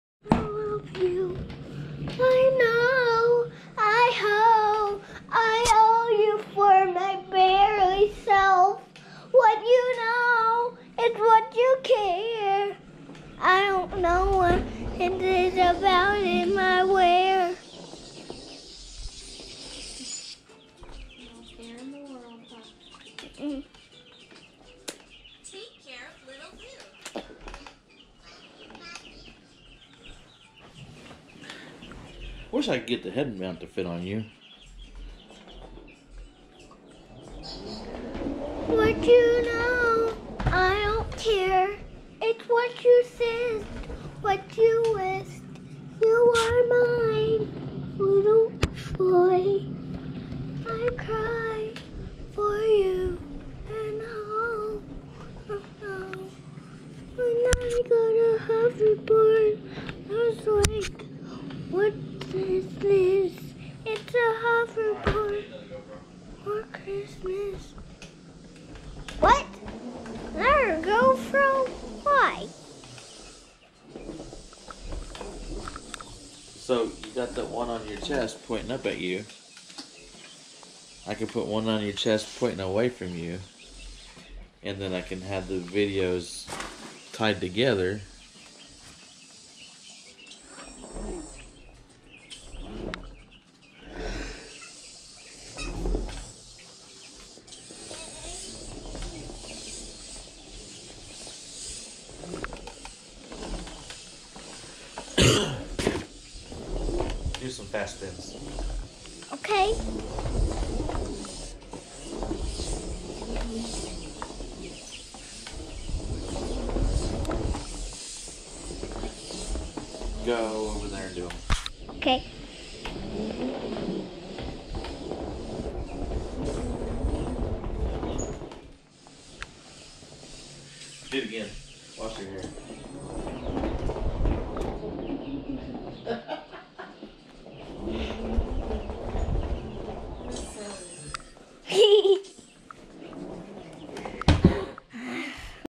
gopro karl
niece on hoverboard still learning to use camera